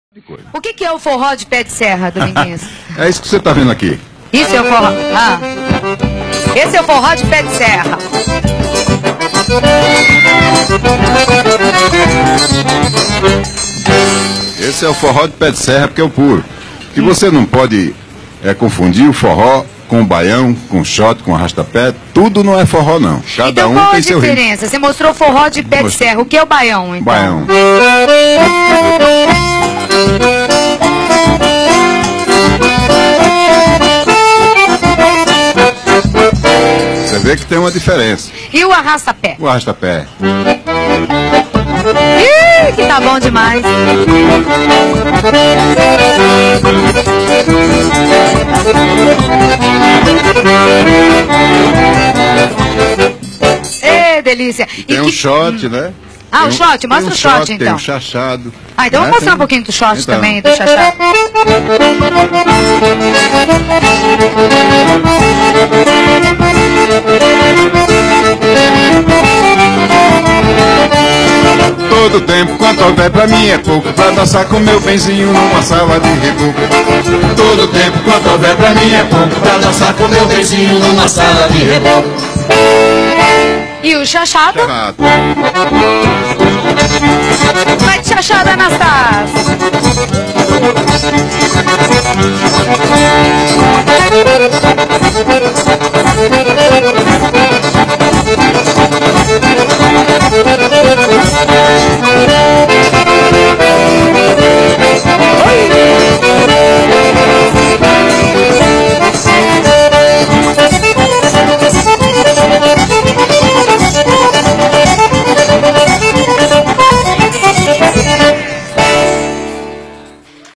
DOMINGUINHOS MOSTRANDO OS RITMOS DO FORRÓ.
Dominguinhos mostra a levada do Forró, Baião, Xote, Arrasta-pé e Xaxado. Reparem que cada instrumento muda pra cada ritmo.

Forró antigamente era a "Festa", depois se tornou um gênero músical.